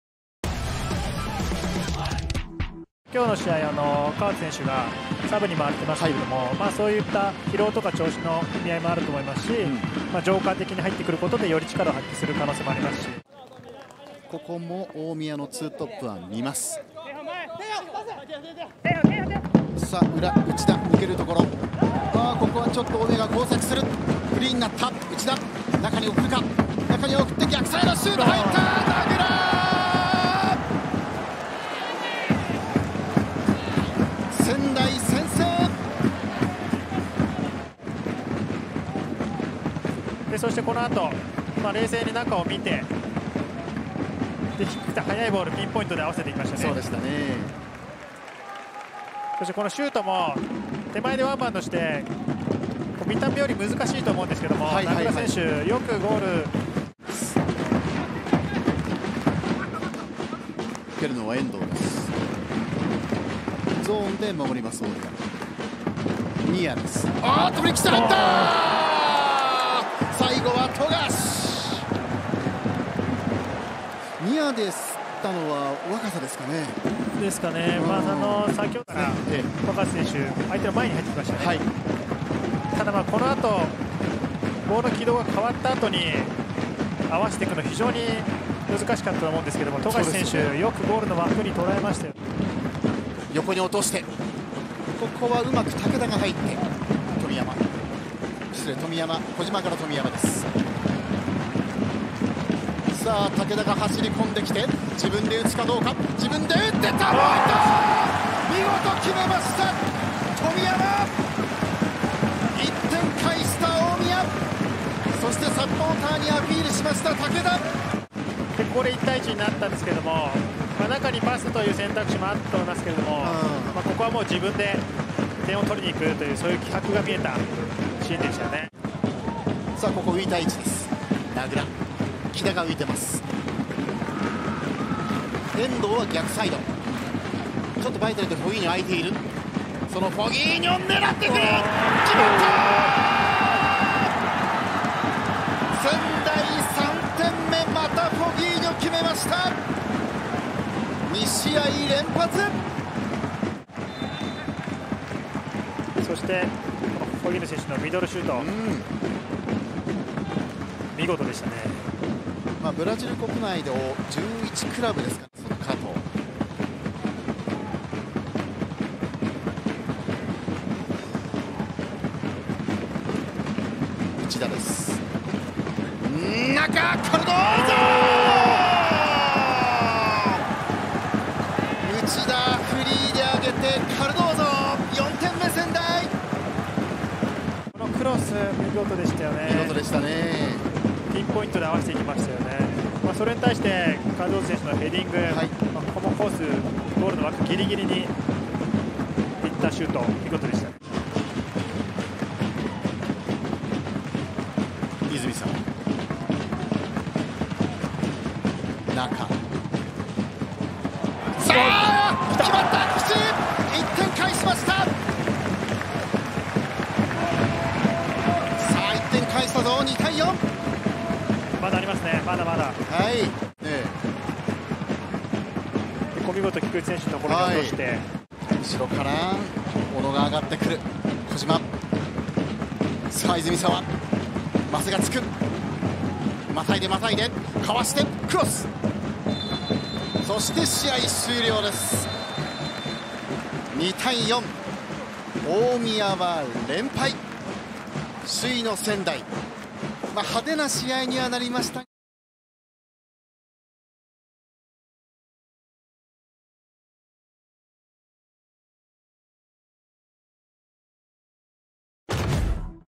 Omiya Ardija 2-4 Vegalta Sendai | MW 17 | 2022 MEIJI YASUDA J2 LEAGUE
Here are the highlights from the rescheduled Matchweek 17 of the 2022 MEIJI YASUDA J2 LEAGUE between Omiya Ardija and Vegalta Sendai.